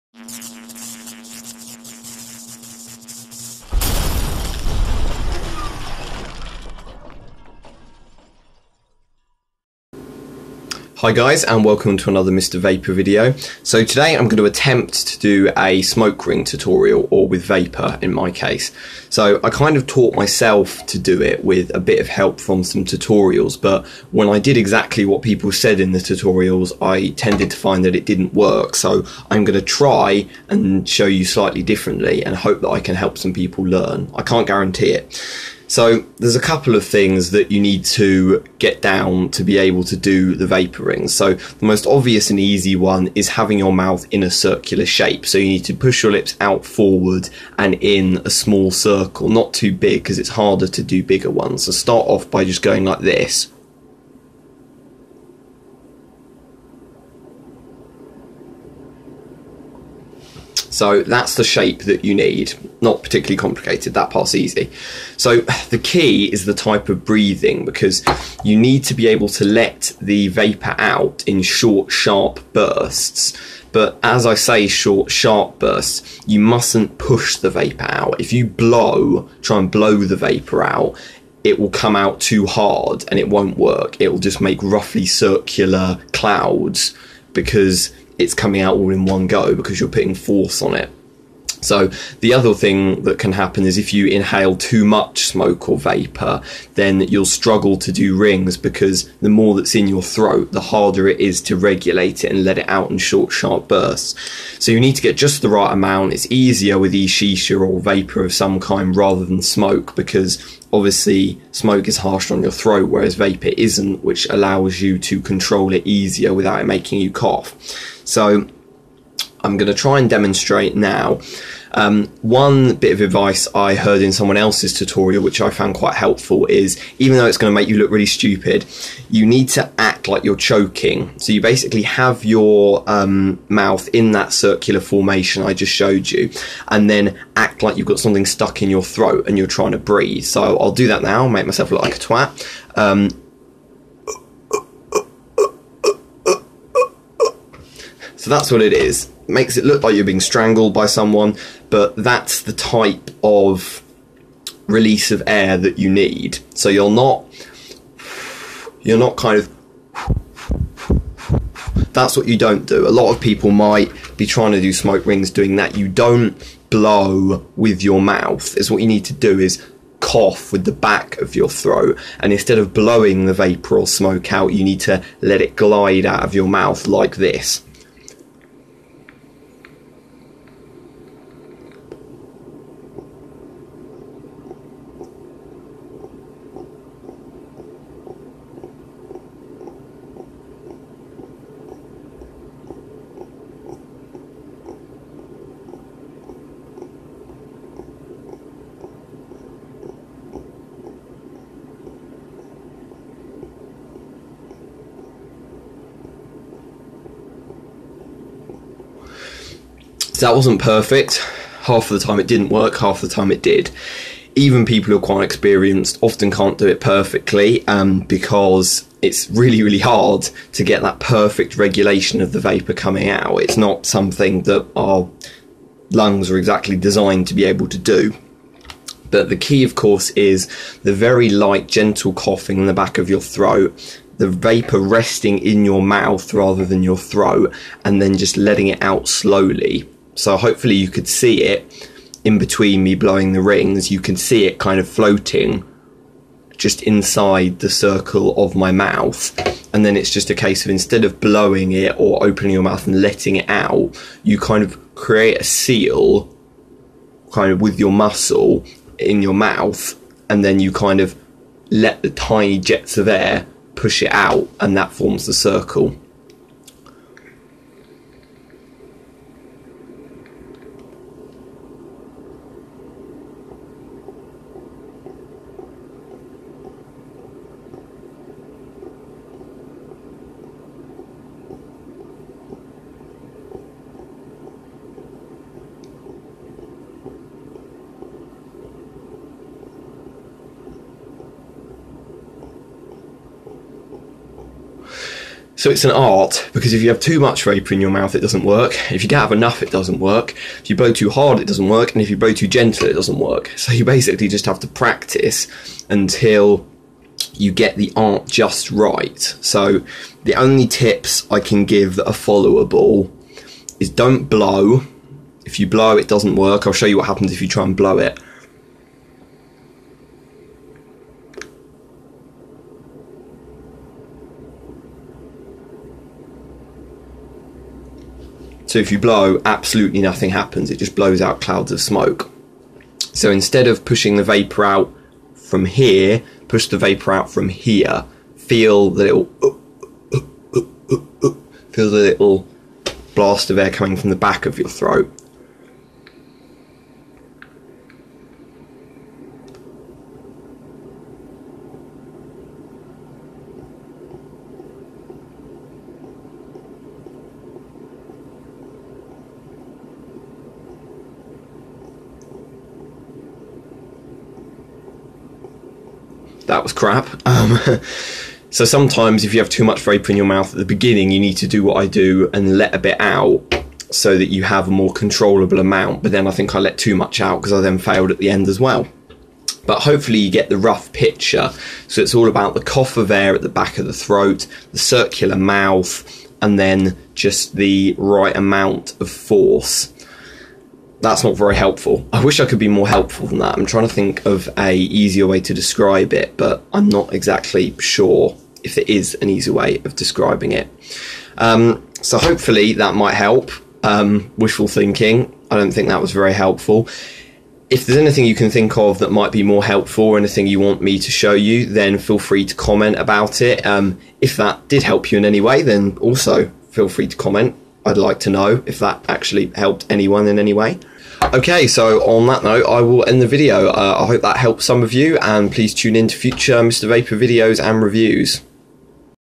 Basic Smoke Ring Tutorial
I try my best to explain the technique behind blowing smoke/vapour rings, I know its not the best tutorial in the world but if its of the least bit of use to anyone then it was worth the effort ;)